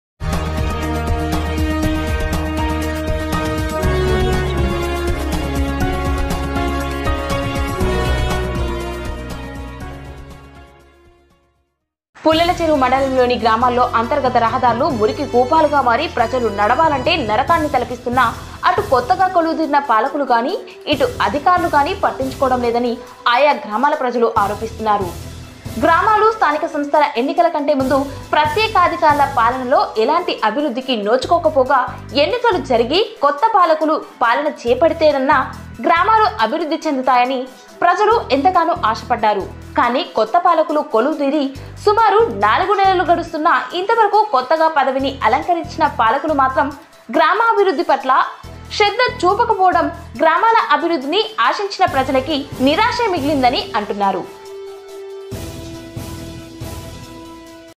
మురికి కుంపాలుగా మారుతున్న గ్రామీణ రహదారులు పట్టించుకోని అధికారులు ||APno1News
మురికి కుంపాలుగా మారుతున్న గ్రామీణ రహదారులు పట్టించుకోని అధికారులు